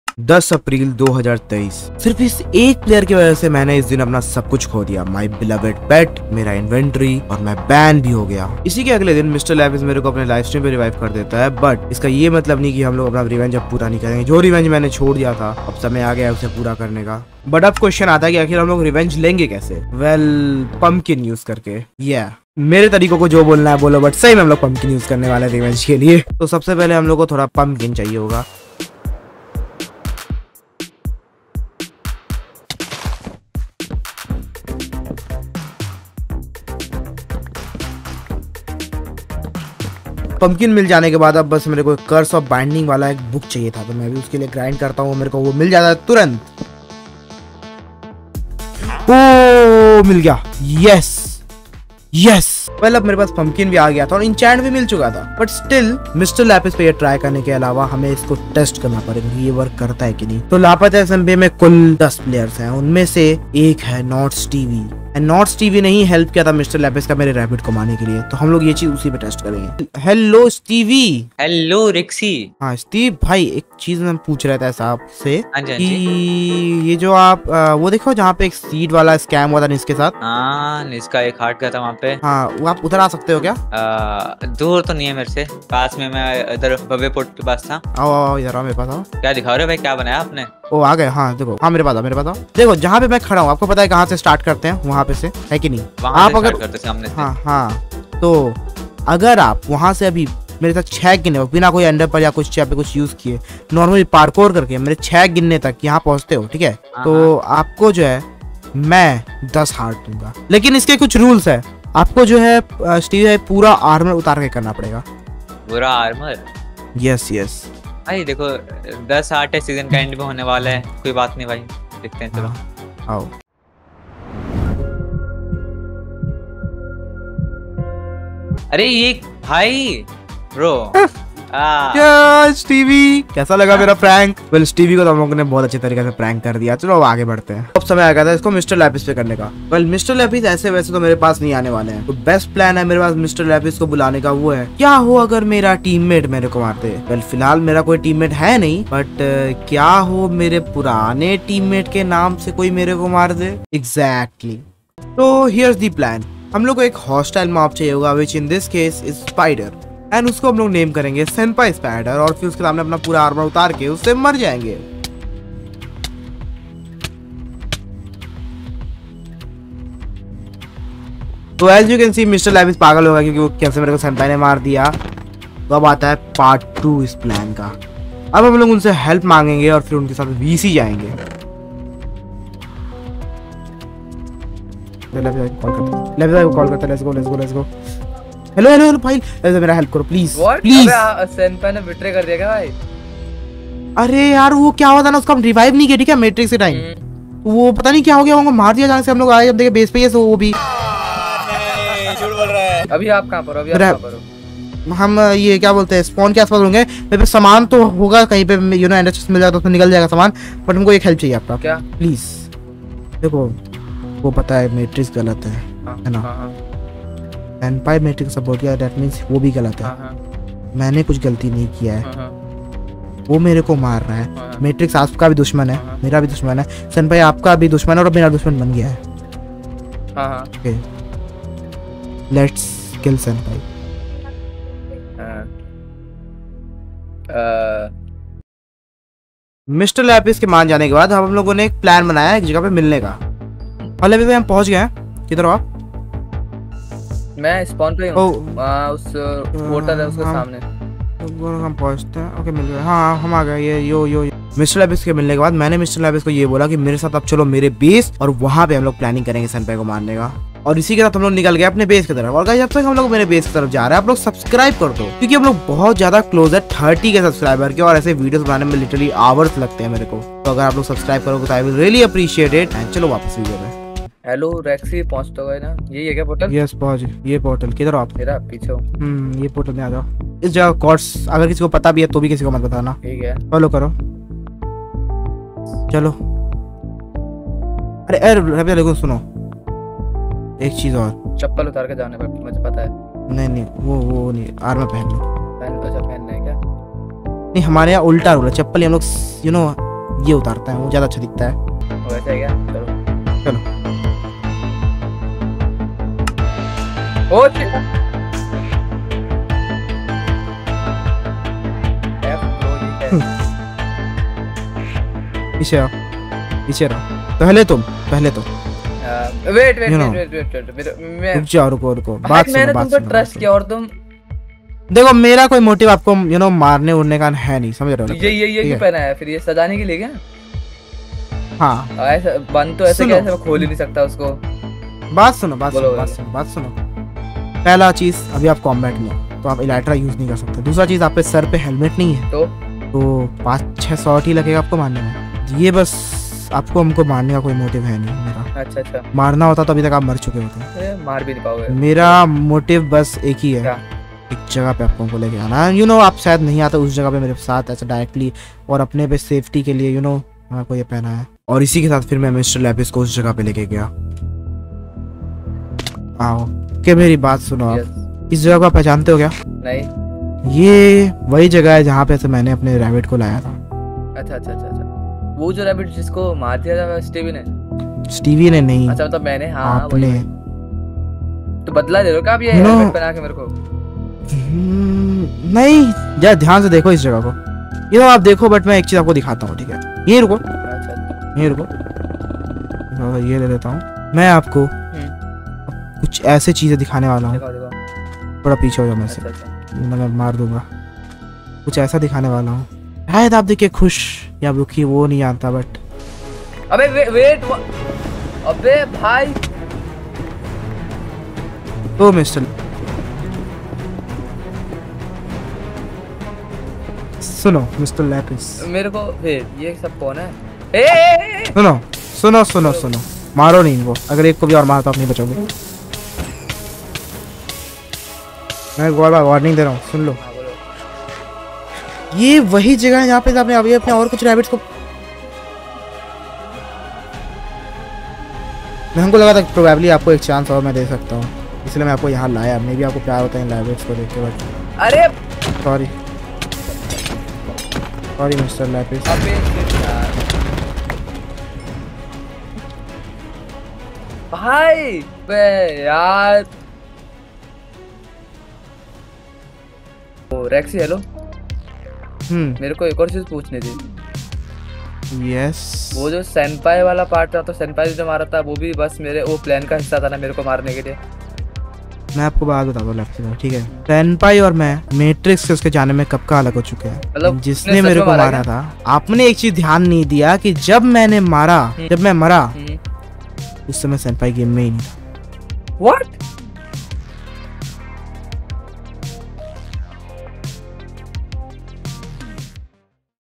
Using Pumpkins to get revenge on my enemies in Lapata SMP
In this video I used pumpkins to avenge my rabbit toast.
watch till the end to find out how!!!